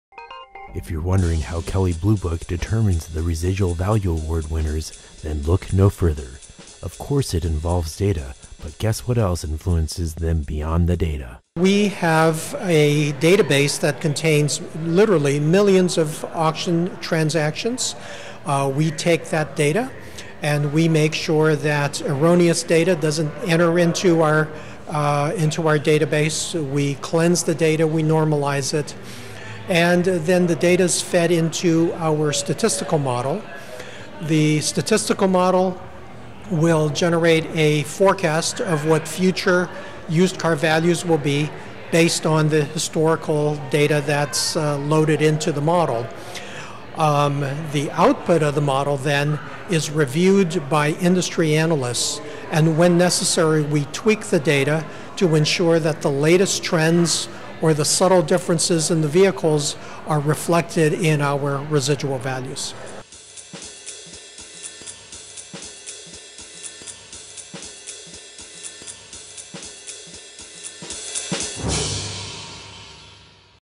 KBB Residual Value Awards Using Data Properly | Eric Ibara | Automotive Digest
If your wondering how Kelly Blue Book determines the residual value award winners, then look no further.  Of course it involves data but guess what influences them beyond the data.

There is a ton of data from auctions, possibly enough to call big data, but how can KBB look at this data and determine the winner of residual value awards.   Panels of experts, data scientist, and another key ingredients go into choosing the prestigious winners of these awards.

Eric Ibara
Director of Residual Consulting
Kelley Blue Book

Ain_11_27_12_KbbAwards_ Eric_Ibara _KBB_VC2.mov